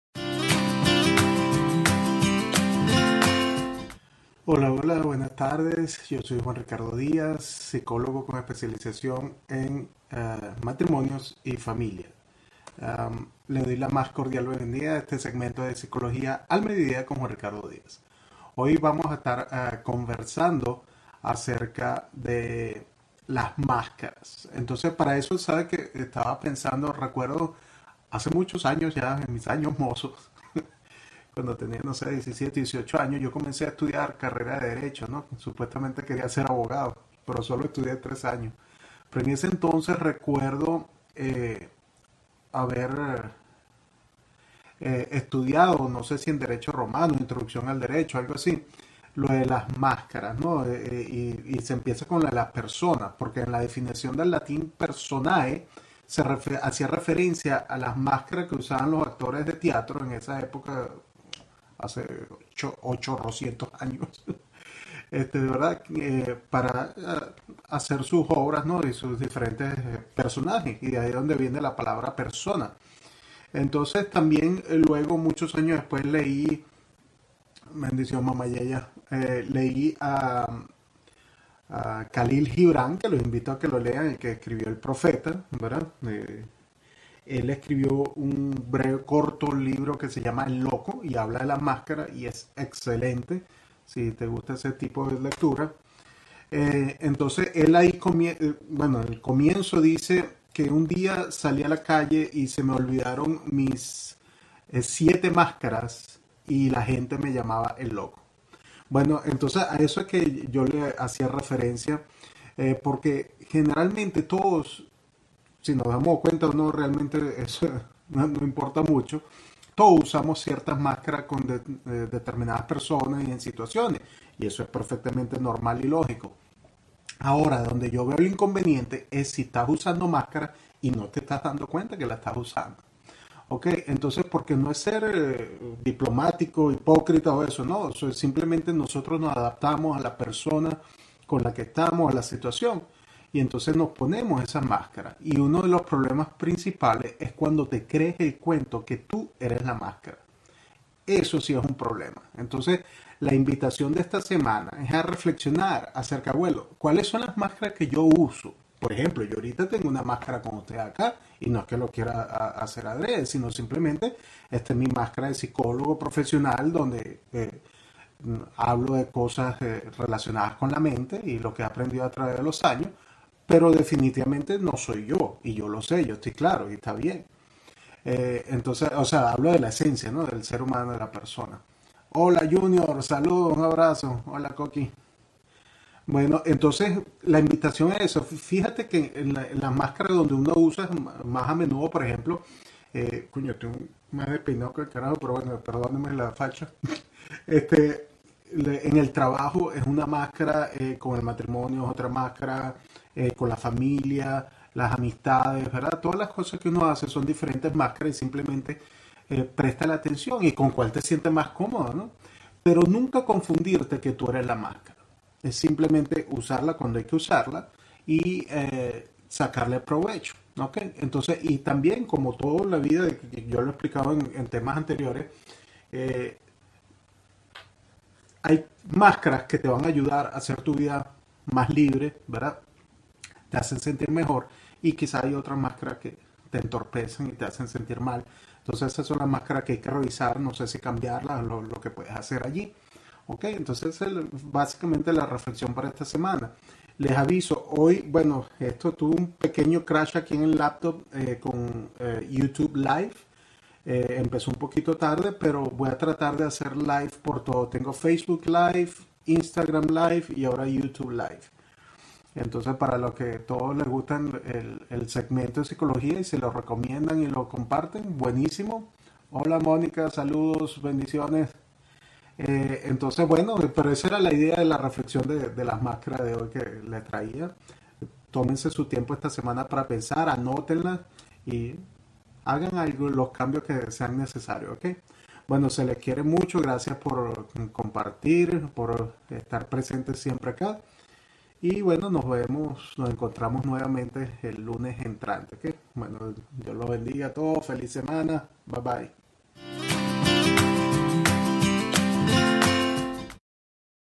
Episodio No.34-Mascaras
Conversando acerca de las mascaras que utilizamos a diario